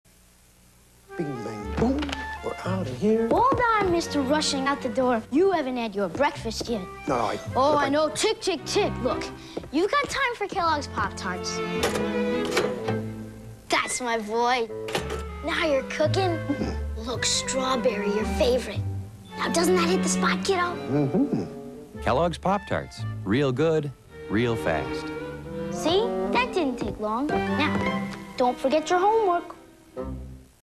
1992 Pop Tarts TV Commercial
The commercial was originally aired 8/12/1992. It was shown during the NBC soap opera "Days of Our Lives" Live broadcast on channel 12 KPNX NBC Phoenix, AZ